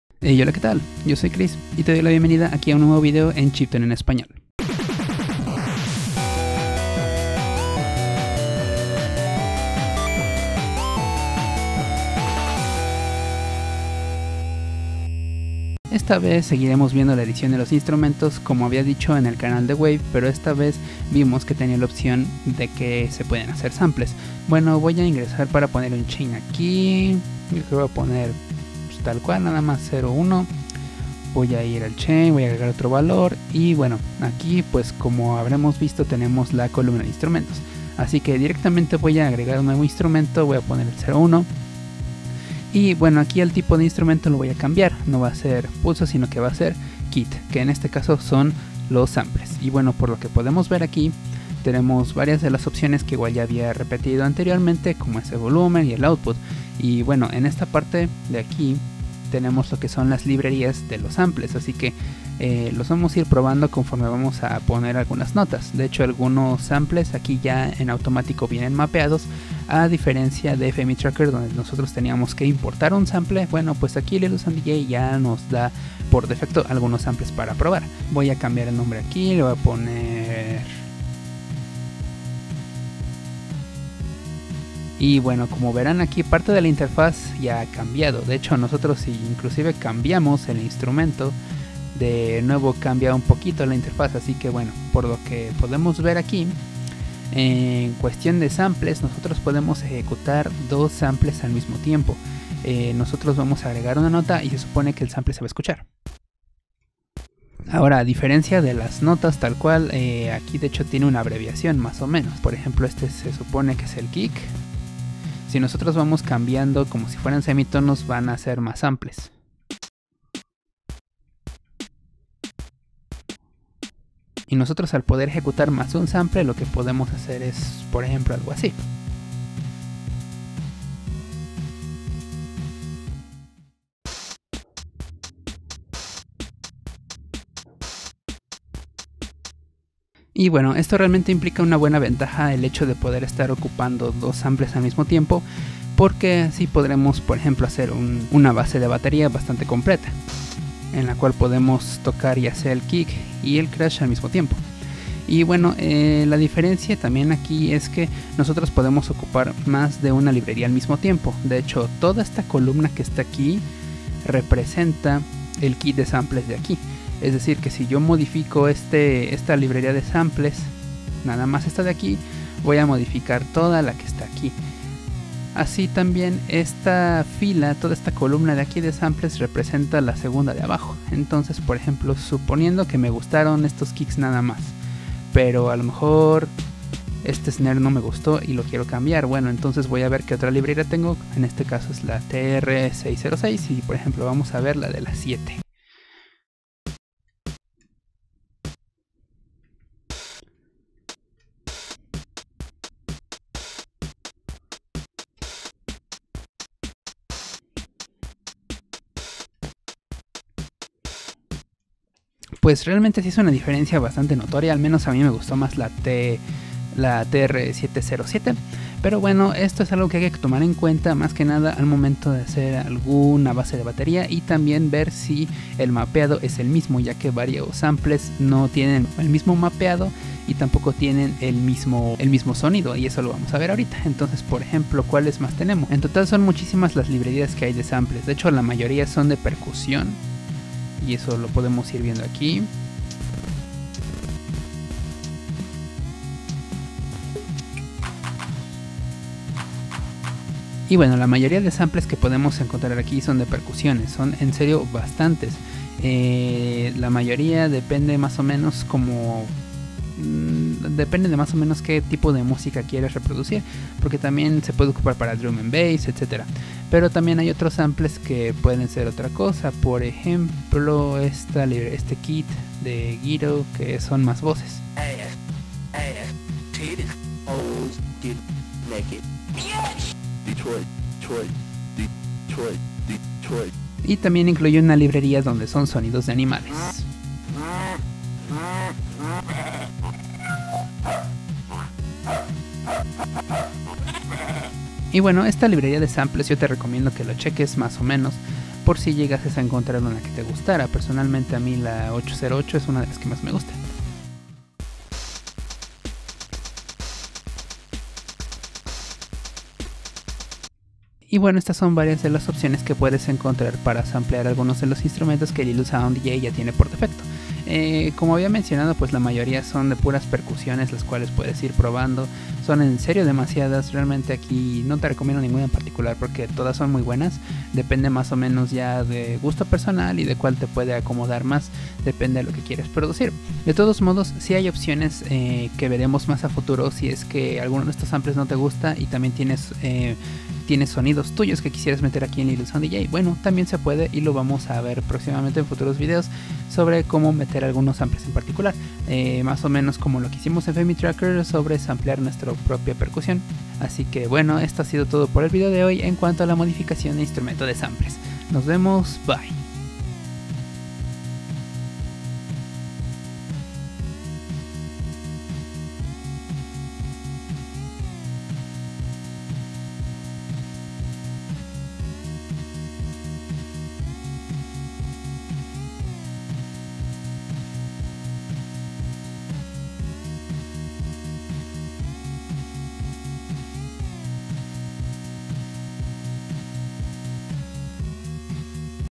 INSTRUMENTOS: SAMPLES - LSDJ - TUTORIAL EN ESPAÑOL - #05
Hoy veremos una de las opciones que LSDj nos da para para producir nuestra música, el cual es la función de sampleo, una función bastante útil para agregar sonidos de batería, voces, etc.